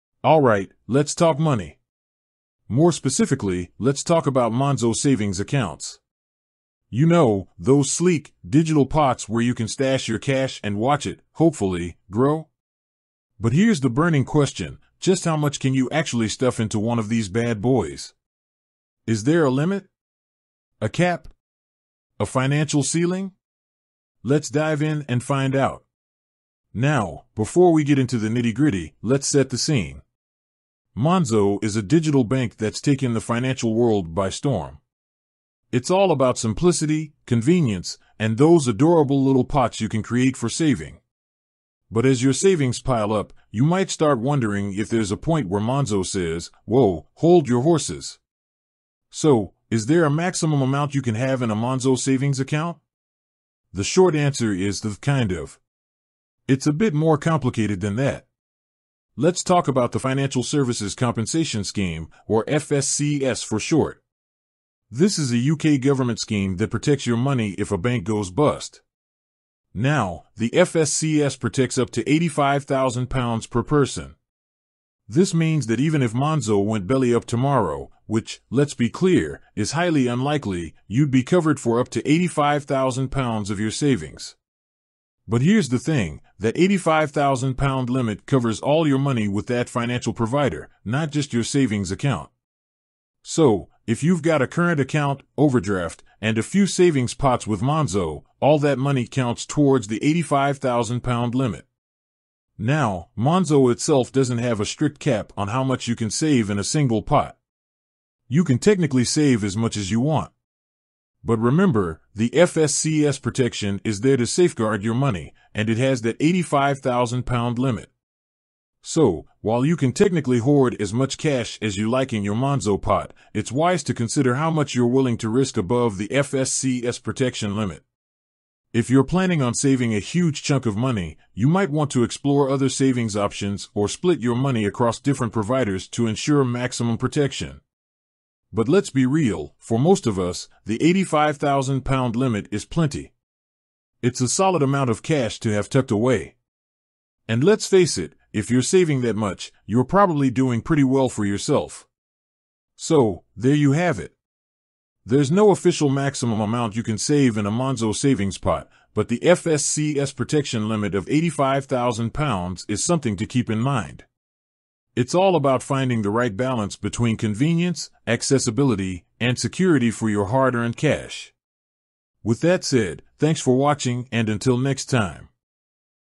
What is the maximum amount you can have in a Monzo savings account
In this video we talk about What is the maximum amount you can have in a Monzo savings account